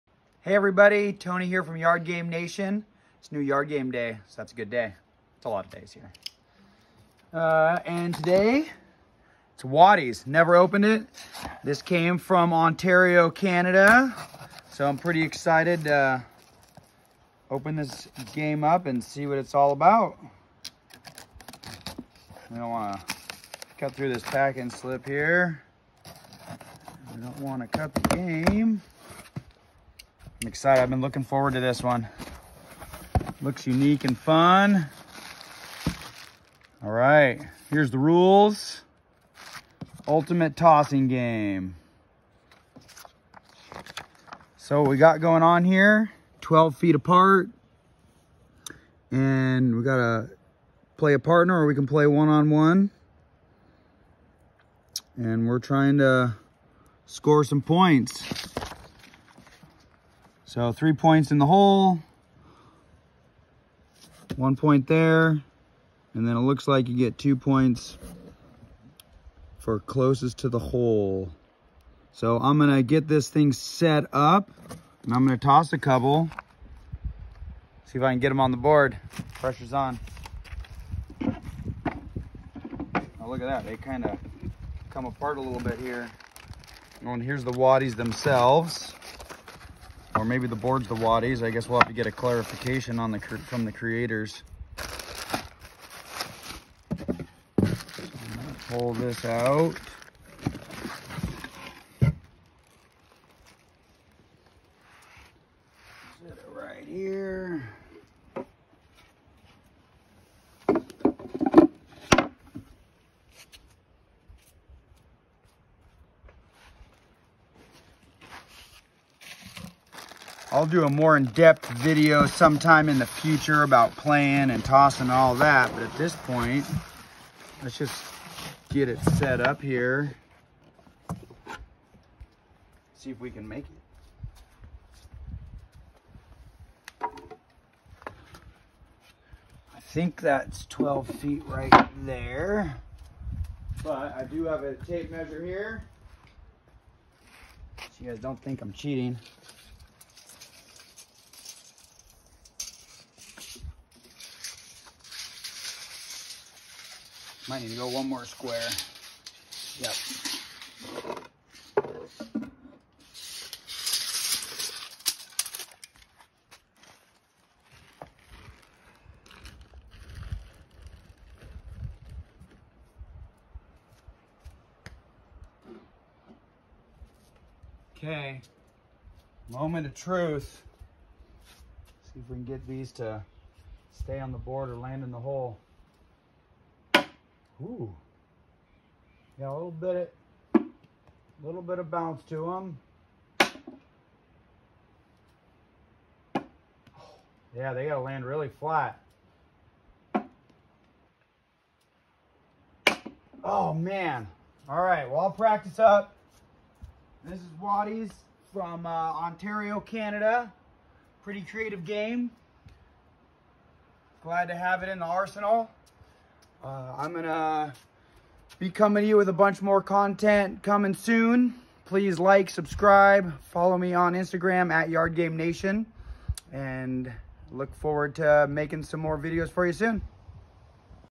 Waddies Yard Game Unboxing
Check out Waddies a new tossing yard game coming out of Ontario, Canada. In this unboxing you will get to see what comes with an order and learn a little bit about how the game is played.